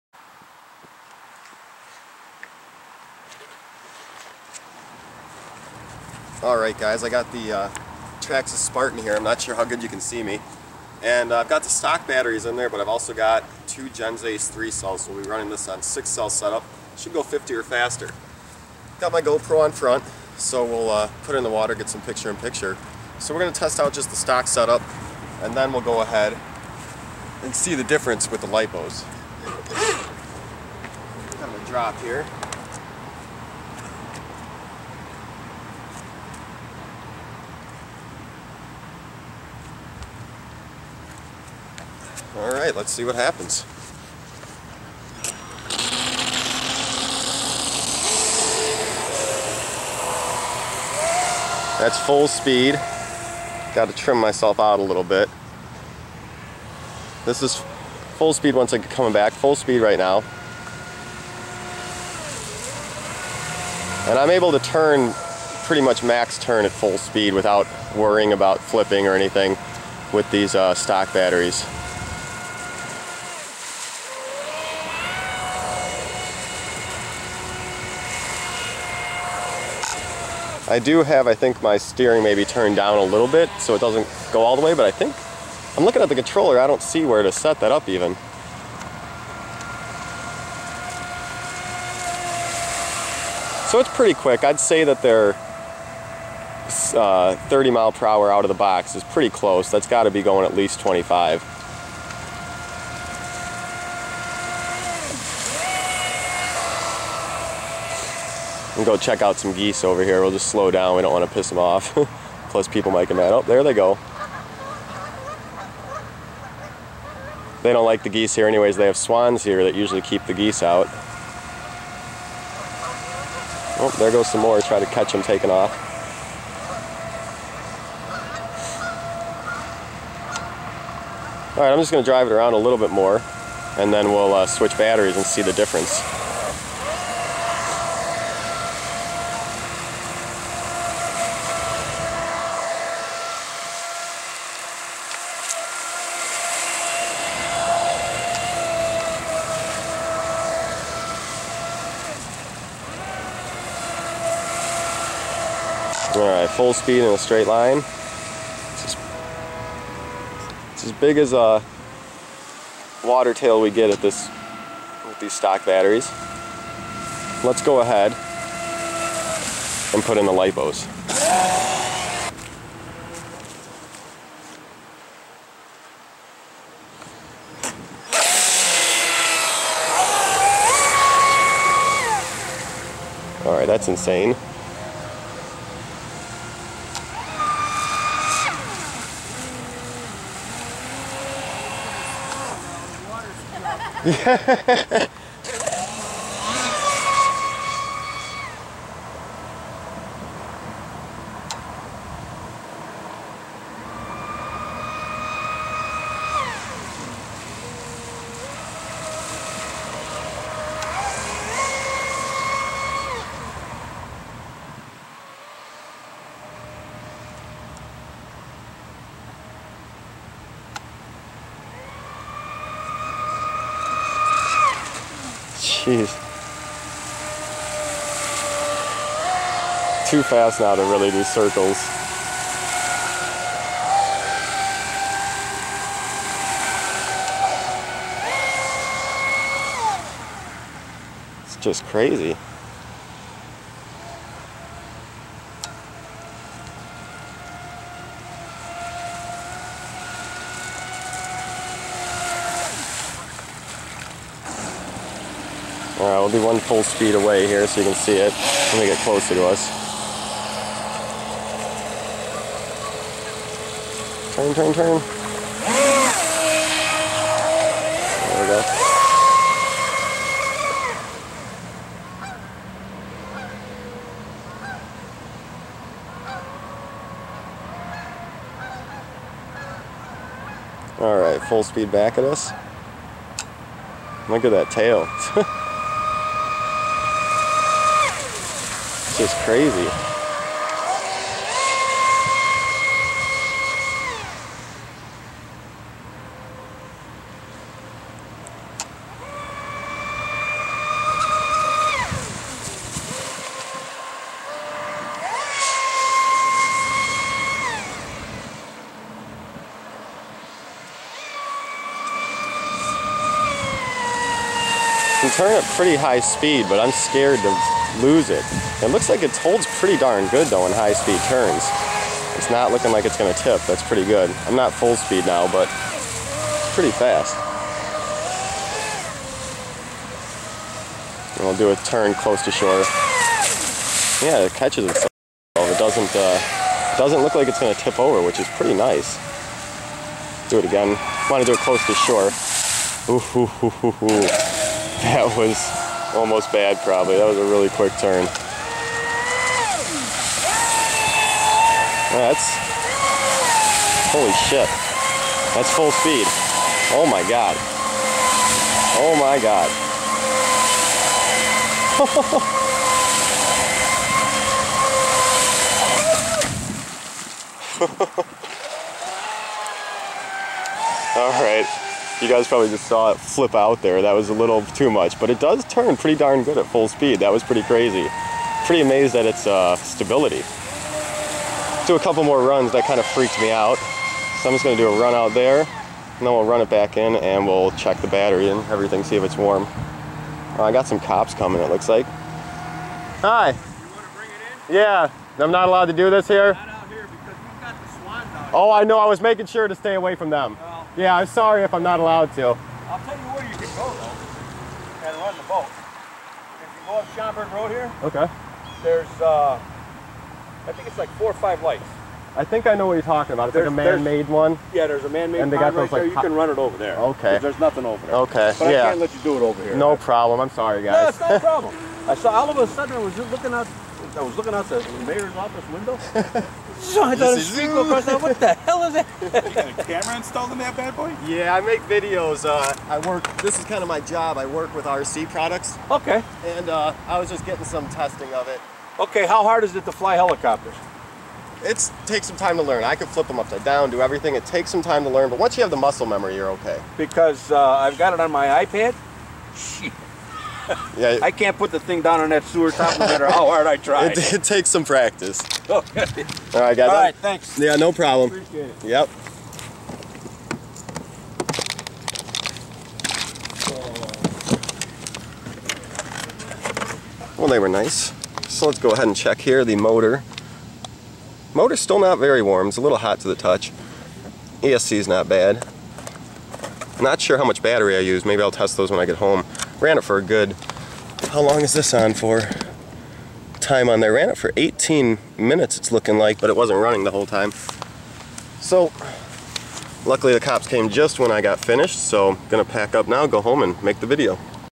Traxxas Spartan - Stock Batteries Vs. 6 Cell Lipo!
I finally got dual 3 cell lipos for my Traxxas Spartan.  Now it is running on 6 cells!  In this video we test the stock setup, then again we test the boat with 6 cells!  Batteries are Gens Ace 25C 5000mAh.